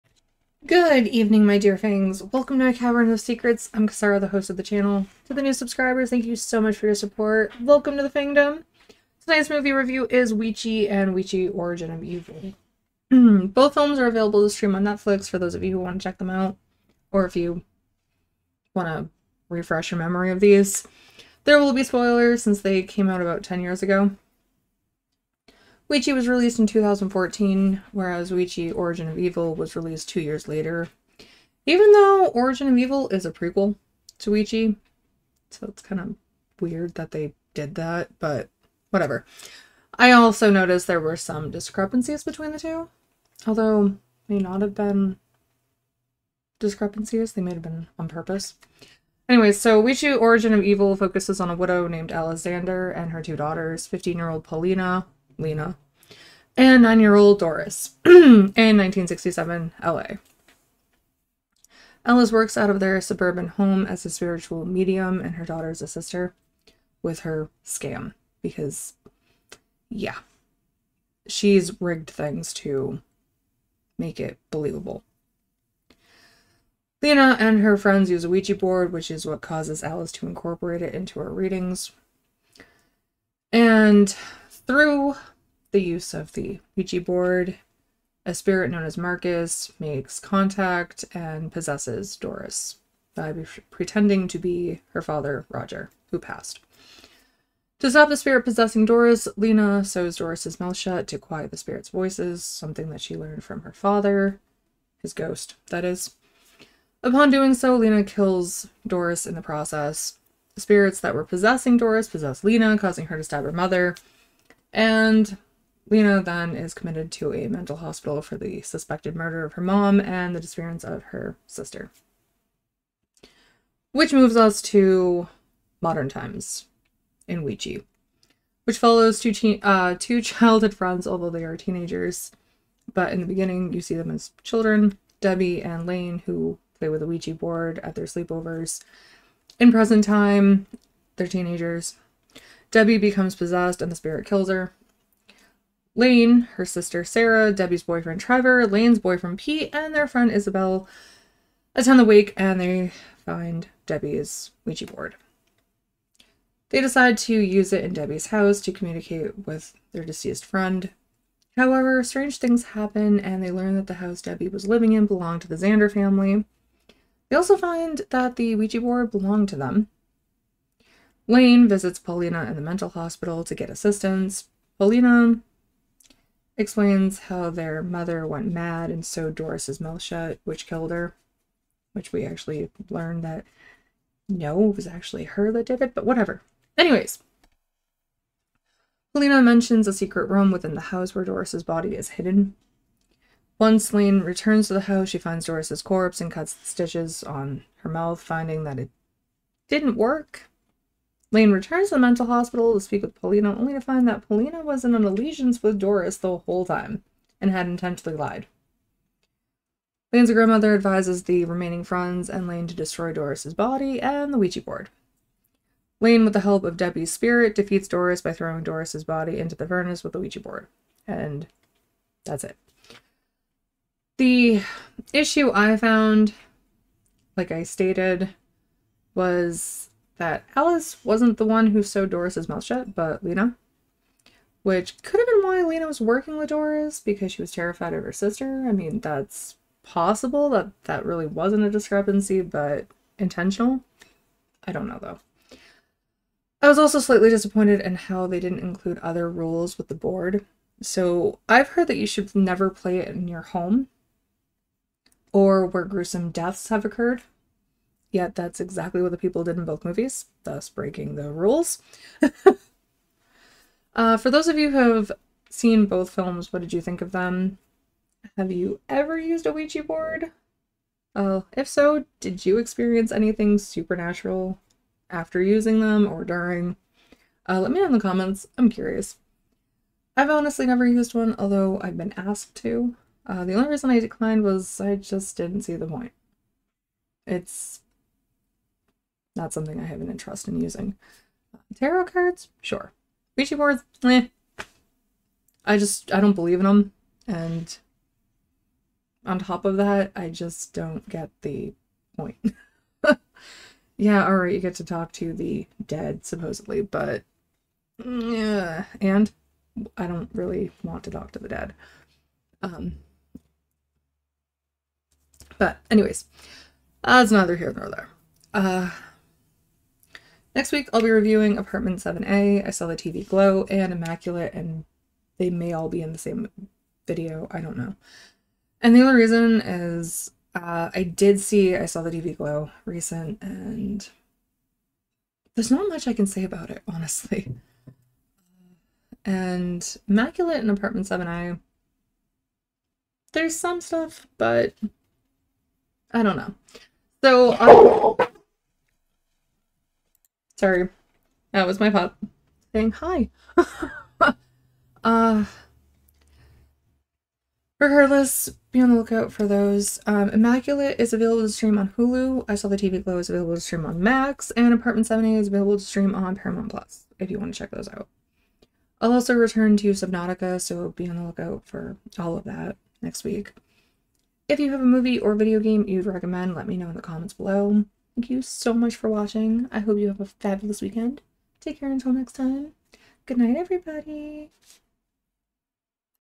Ouija & Ouija: Origin of Evil Reviews (SPOILERS)
Both films are available to stream on Netflix.